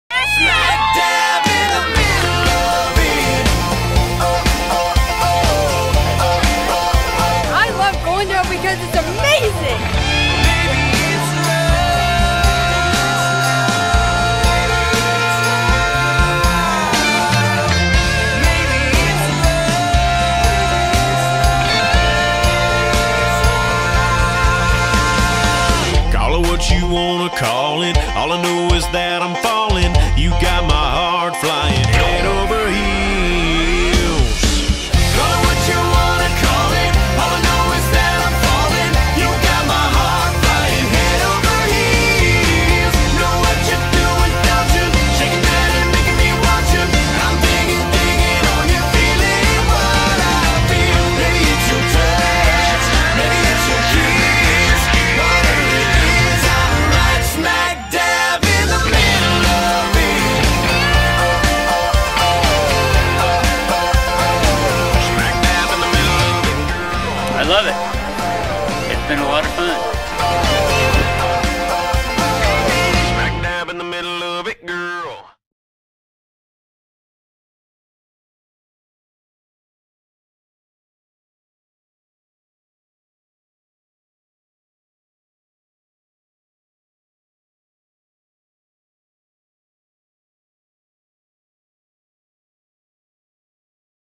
Experience Goldendale! ~ Harvest Festival 2017
A fun video we did for The Goldendale Chamber of Commerce back at the 2016 Harvest Festival.
Klickitat County is home to a vast array of amazing food, drink, fun friendly people and some of the wildest roads and trails we have ever seen.